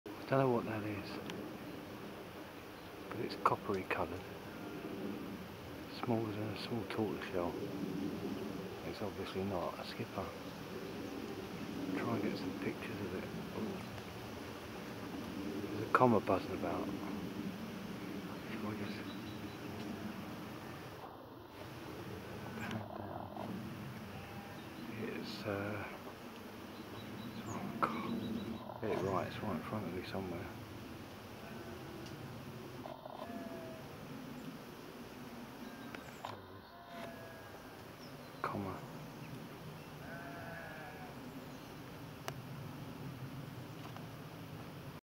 COMMA AND GATEKEEPER BUTTERFLIES
Finally I get all the butterflies I have been seeing with COPPER COLOURING but no Copper Butterflies as I suspected. Nor have I seen any Fritillaries as yet.

Meadow Browns, Commas and now this Gatekeeper along with Small Tortoiseshells all of which abound making burnt orange and copper all the fashion in nature right now.

Only broken up right now by the Ringlet and Large White now and then!